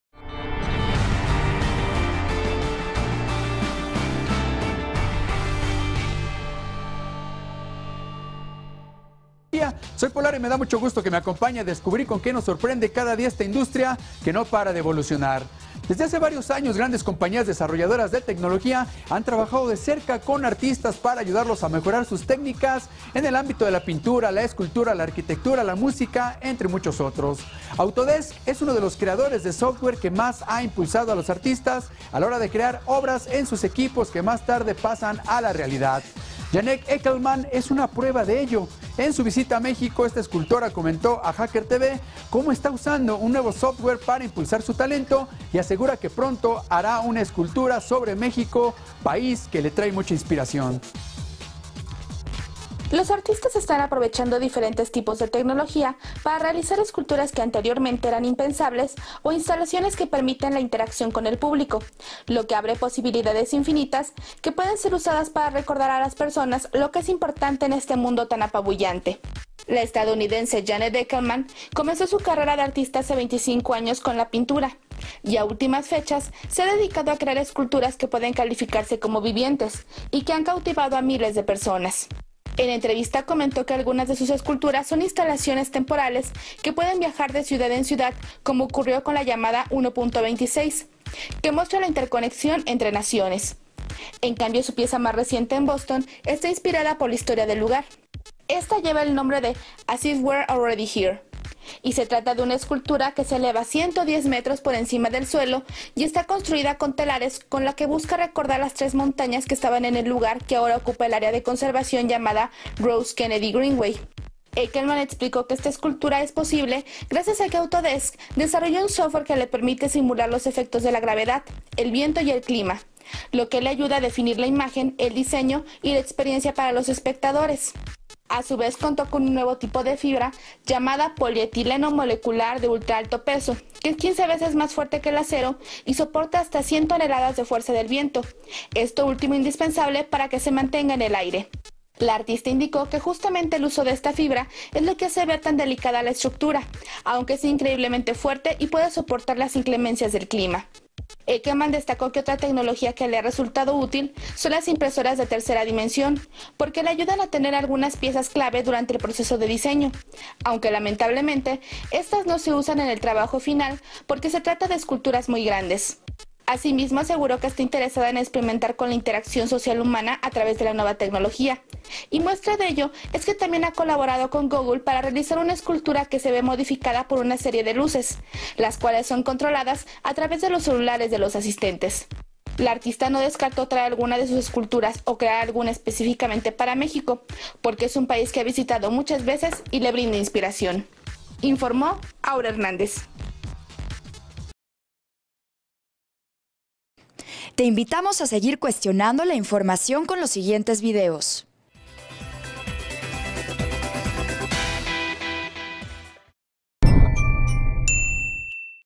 Estructuras gigantes, parte del arte urbano/ Hacker
Se espera que la artista próximamente realiza una obra de arte en la ciudad de México.

08 de junio de 2015